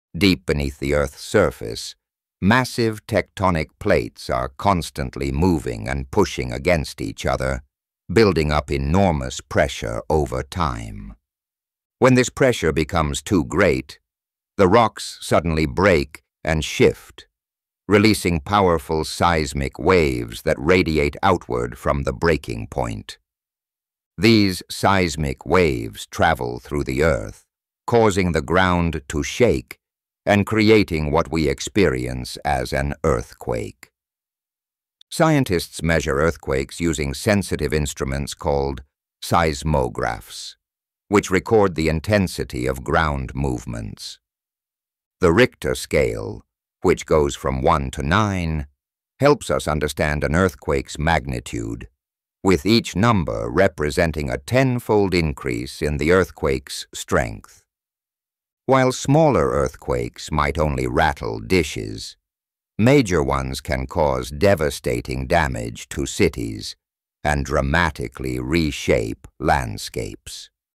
Earth's Hidden Power: Understanding Tectonic Plates and Earthquakes 🌍💥
Dive deep into the Earth's layers and discover how tectonic plates interact beneath our feet! 🏔️ As these massive formations push against each other, they build unseen pressure that can lead to powerful earthquakes. In this video, we visualize the fascinating journey of seismic waves and explore how scientists measure and scale these natural phenomena using the Richter scale. 📊💡 Learn the difference between minor and major earthquakes and their impact on our world. Join us for this eye-opening experience!